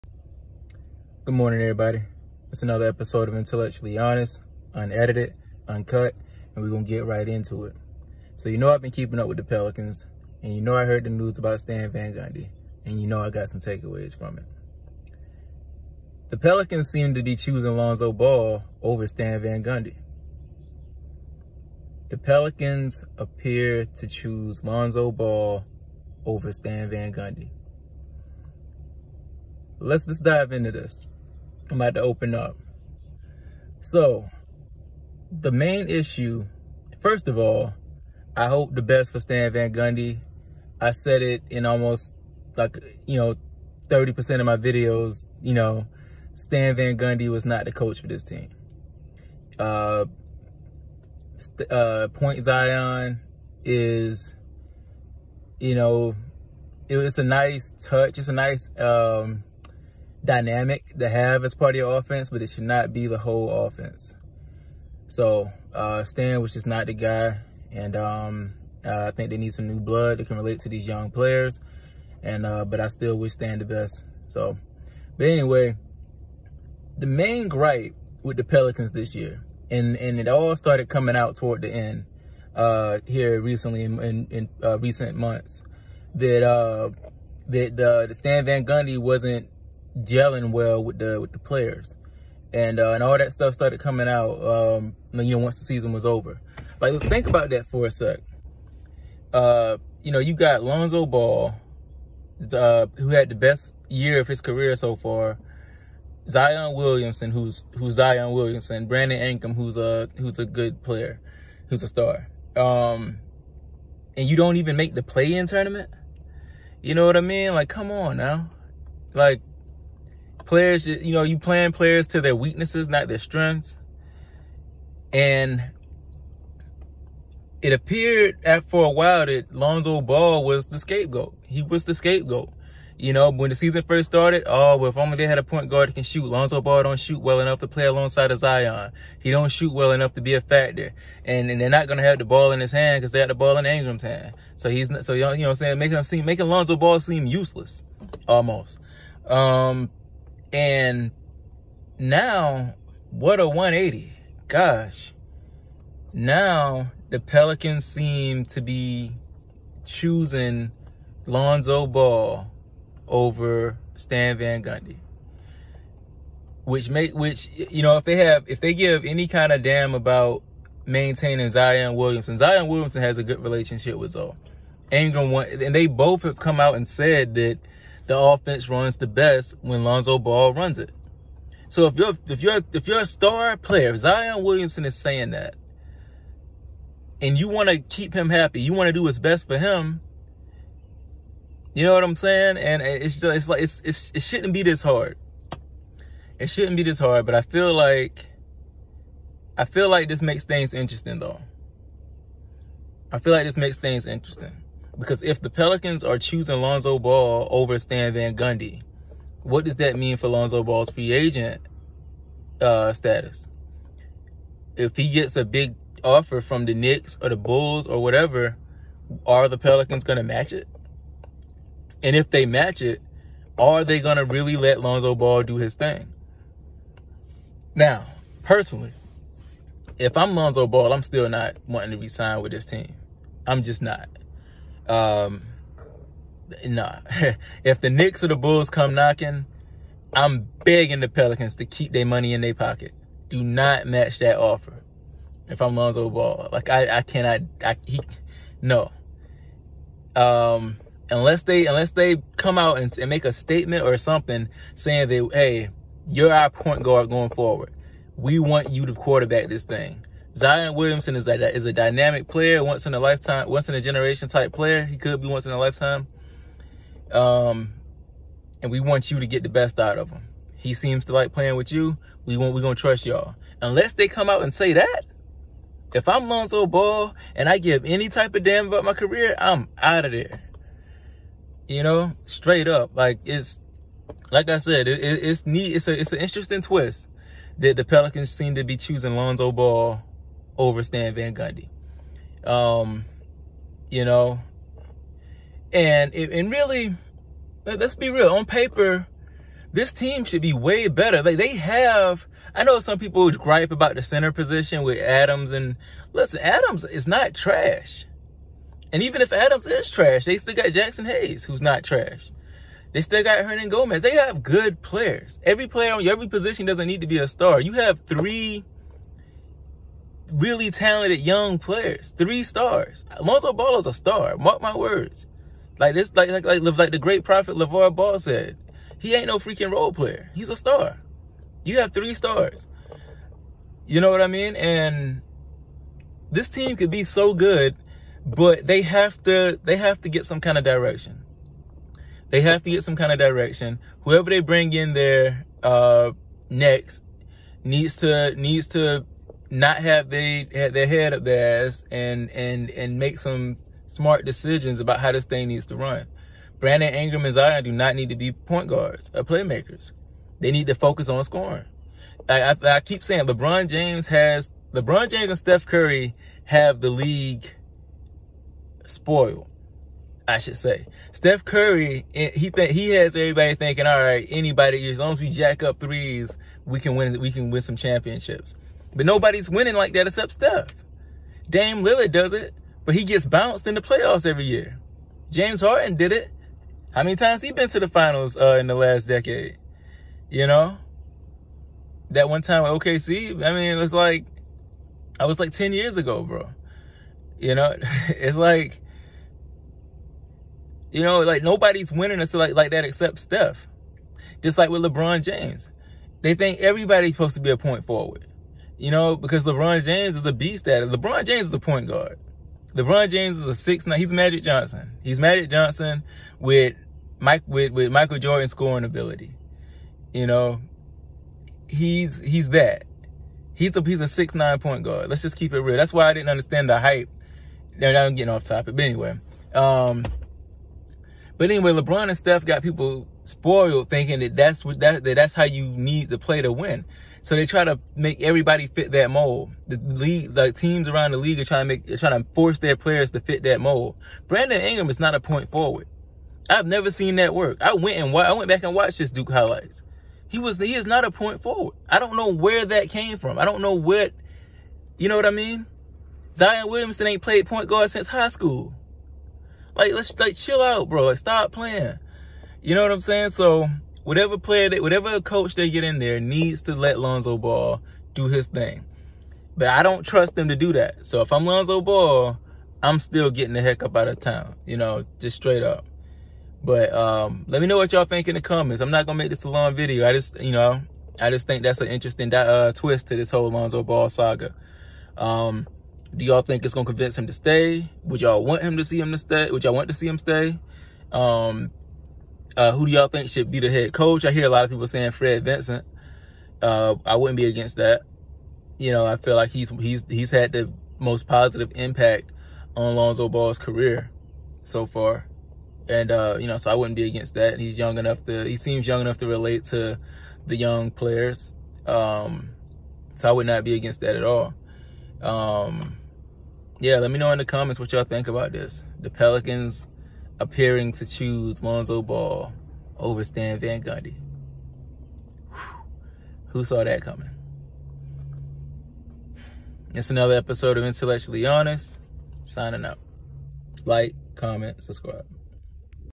Pelicans chose Lonzo Ball over Stan Van Gundy
Pelicans fire Stan Van Gundy to keep Lonzo Ball.